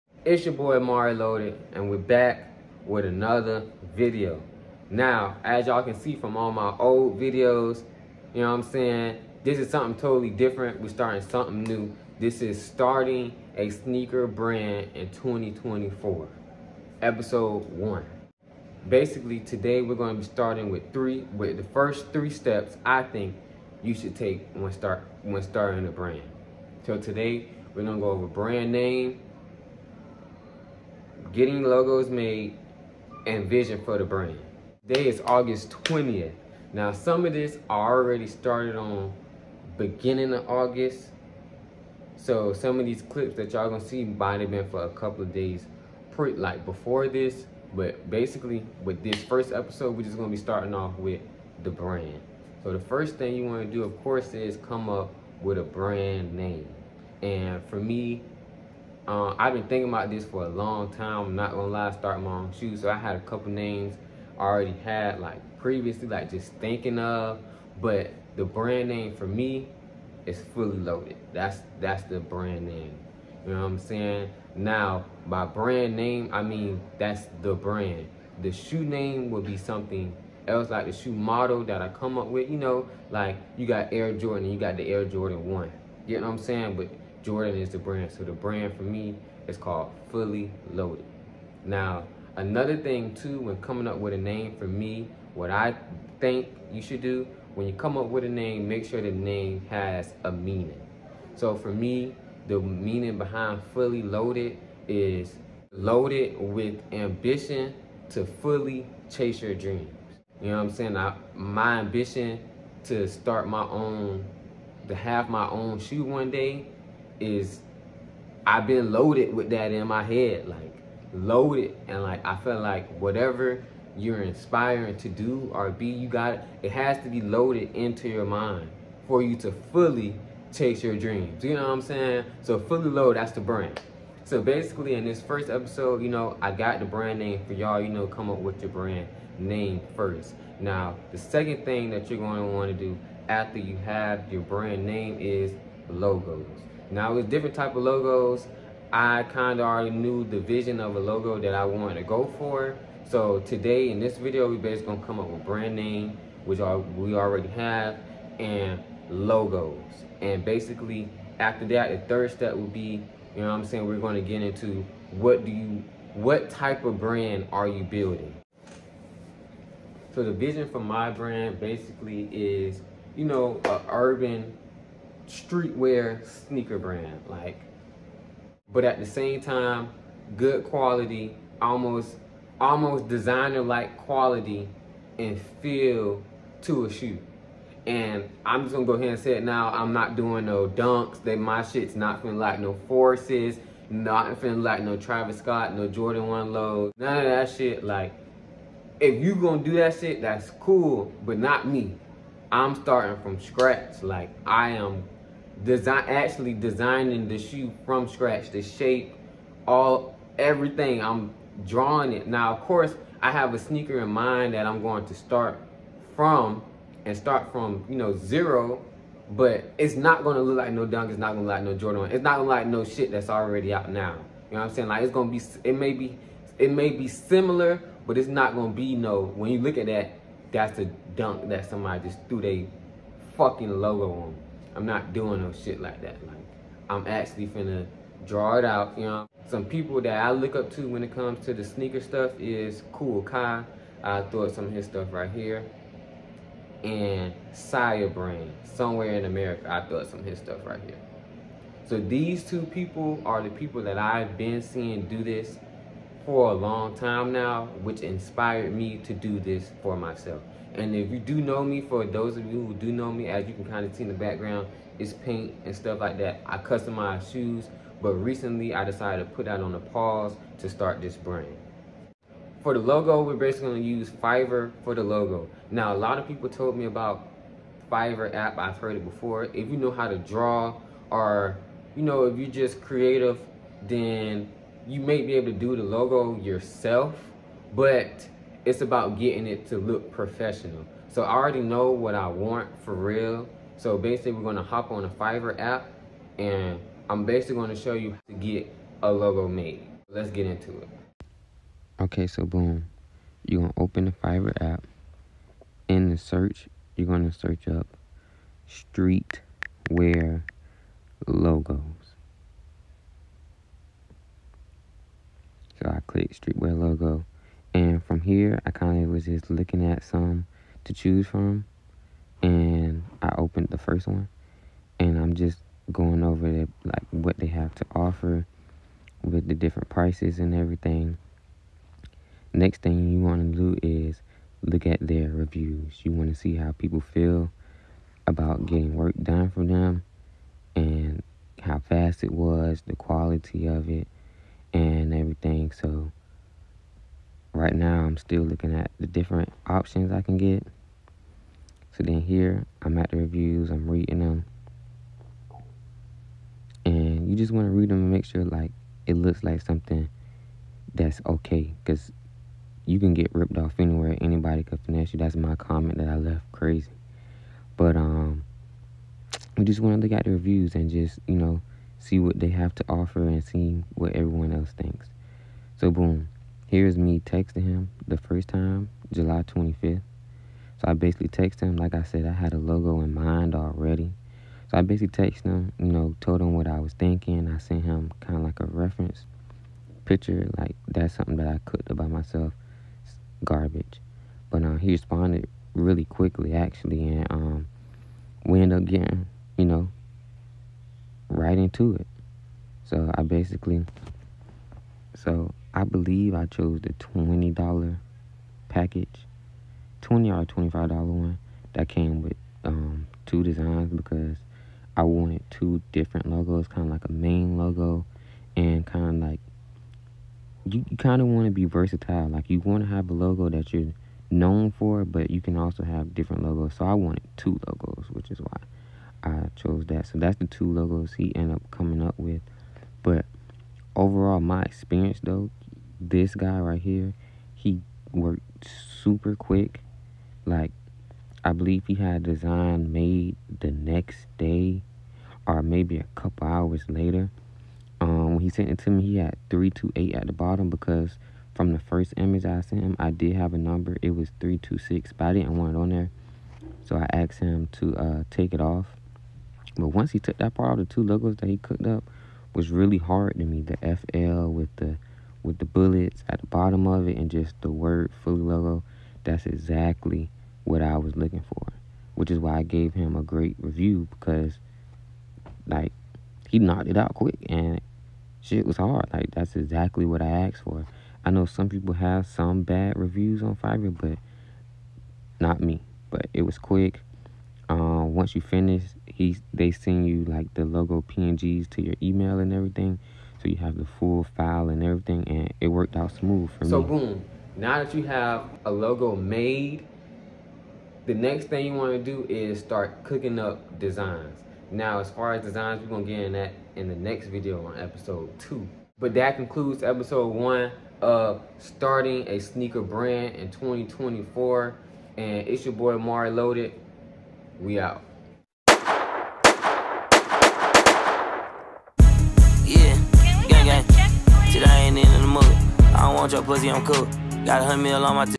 Starting A Sneaker Brand: Episode 1 Brand Name And How To Make A Logo
Episode 2 Drops Saturday, finding a manufacturer.
Follow the brand on all platforms @fullyloaded.__